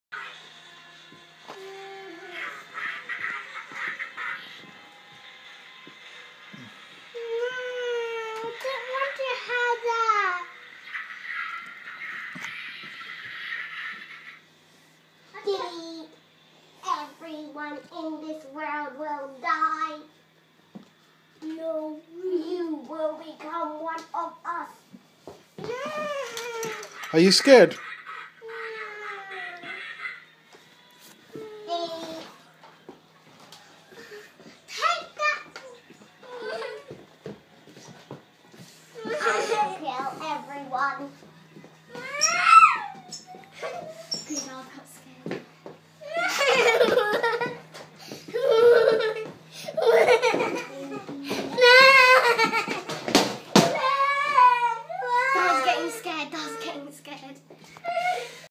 Cyber chaos with A##### and D##$#
Alisha is scaring Dawud with her cyberman mask